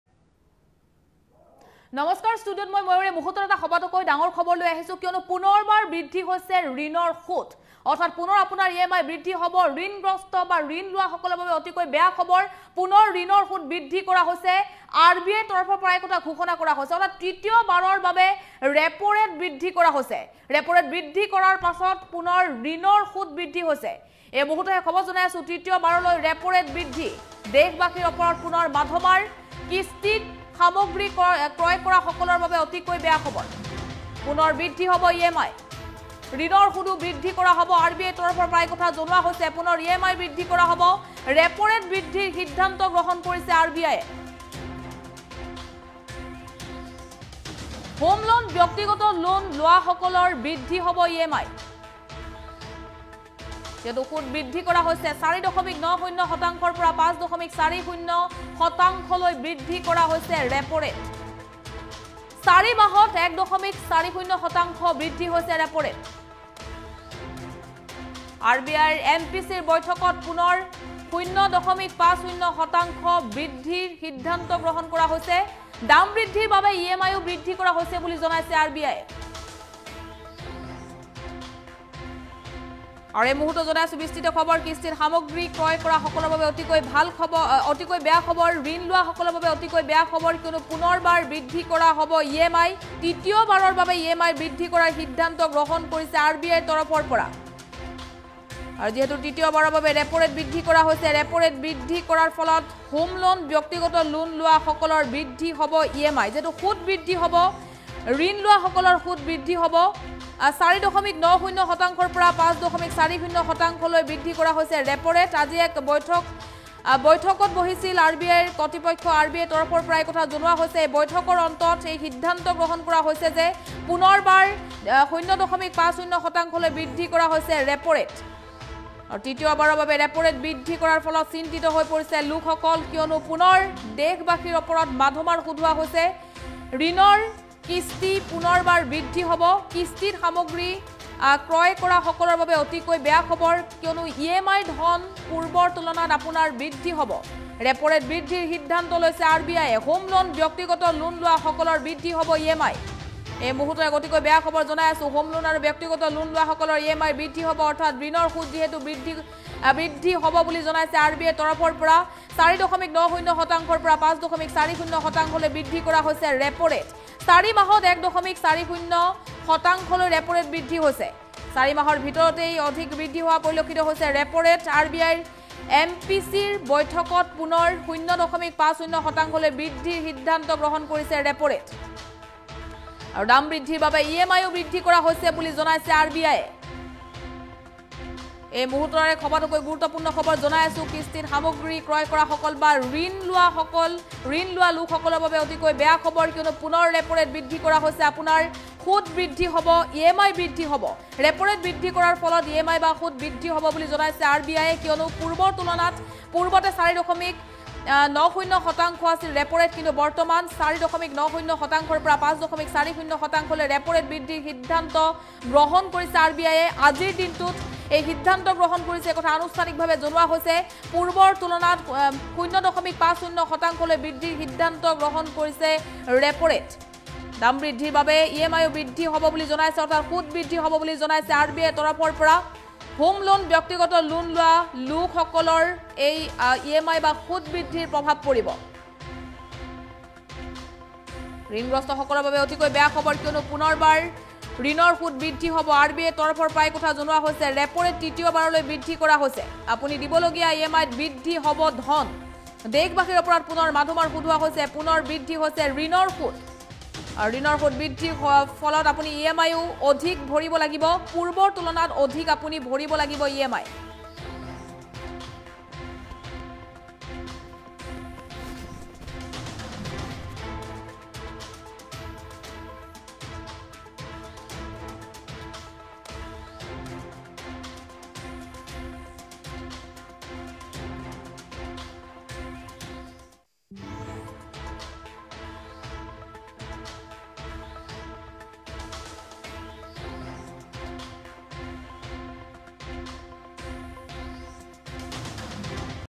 পুনৰ বৃদ্ধি হ'ল ঋণৰ সূদ। তৃতীয়বাৰৰ বাবে ৰেপ'ৰেট বৃদ্ধি। আপুনি দিবলগীয়া EMIত বৃদ্ধি পাব ধন।
পুনৰ বৃদ্ধি হ'ল ঋণৰ সূদ। দেশবাসীৰ ওপৰত মাধমাৰ। তৃতীয়বাৰৰ বাবে ৰেপ'ৰেট বৃদ্ধি। আপুনি দিবলগীয়া EMIত বৃদ্ধি পাব ধন।